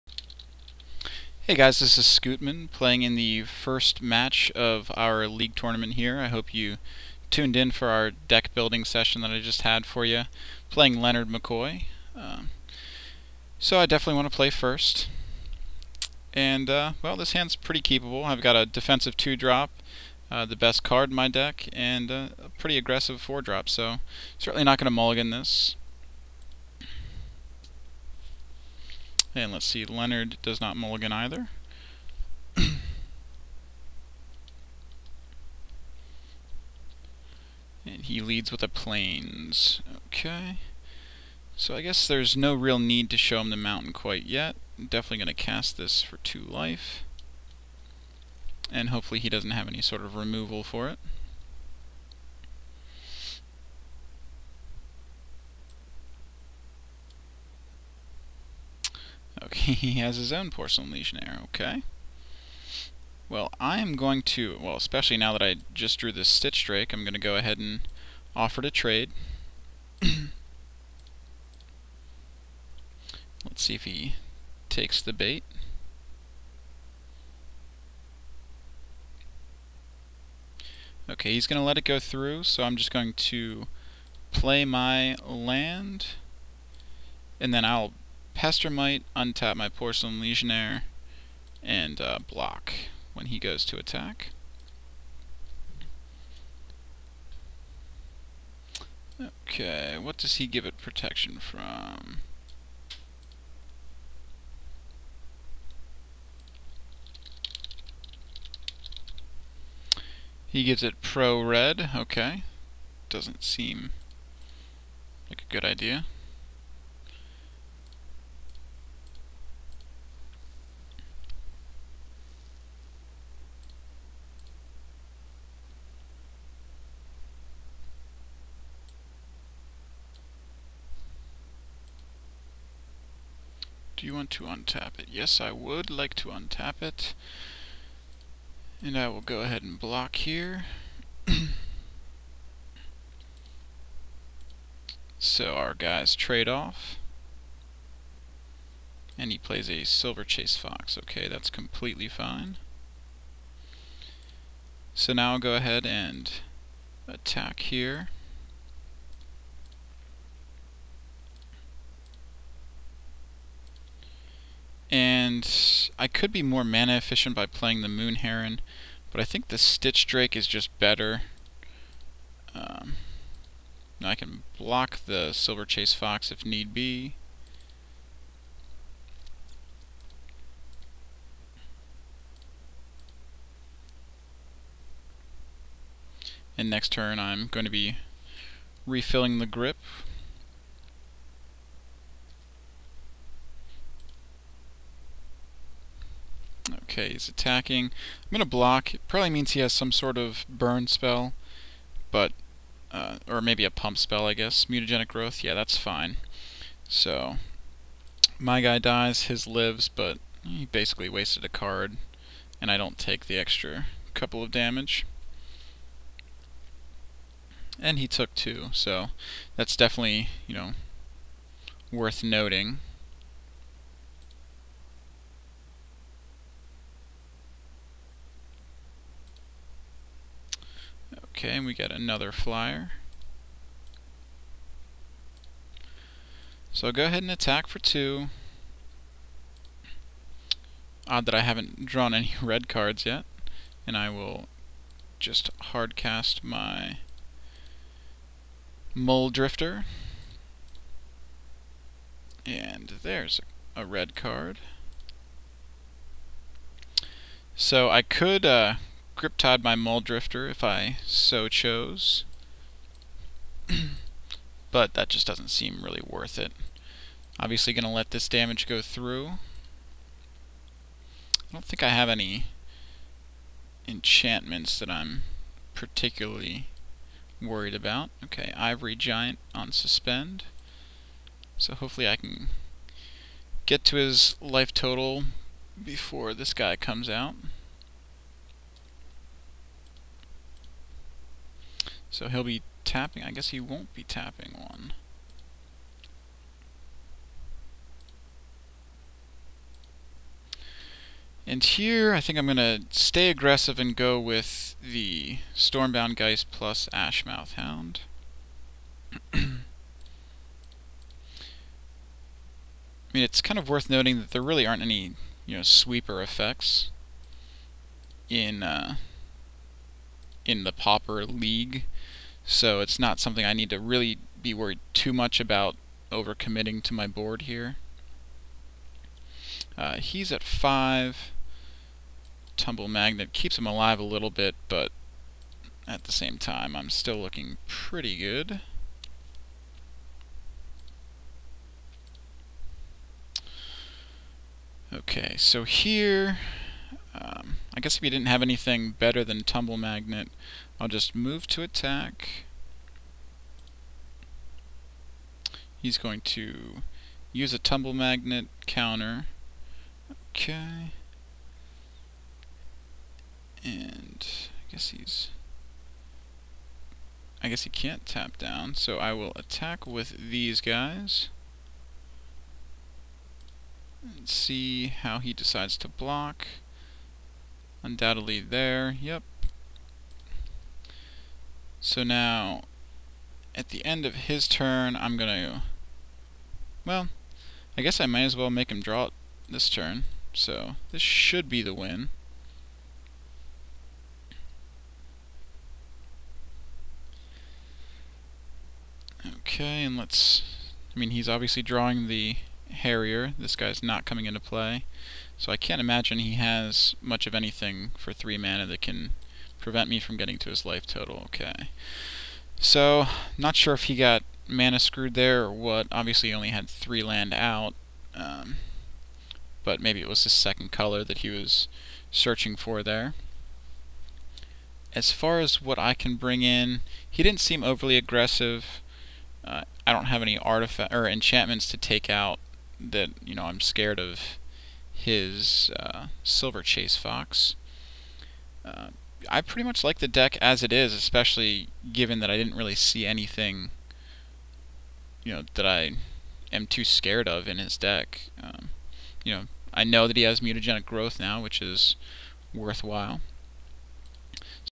Channel Scootmin Seraphim League Match1.1.avi
Scootmin plays Match 1, Game 1 against Leonard_McCoy.